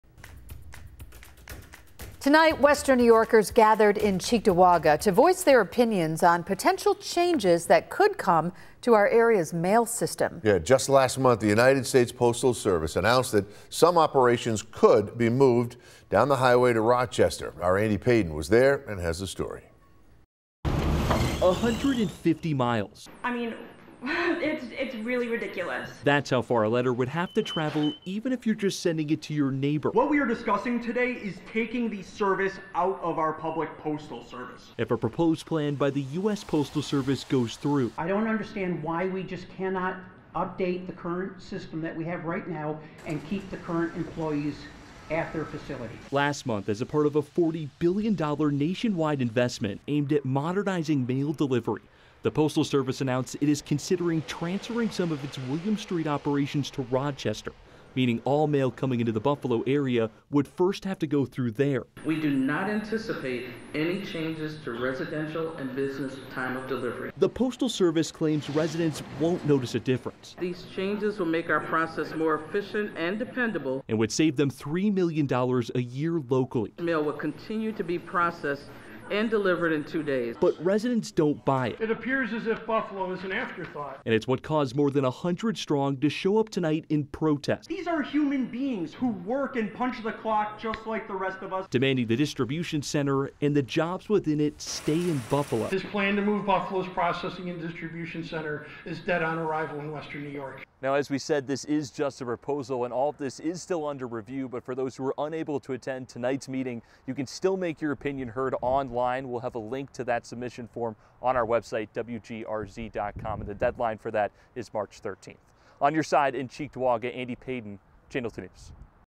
The US Postal Service announced that some operations currently in Buffalo could be moved to Rocheste
A meeting was held for the public to voice their opinions on the Postal Services proposal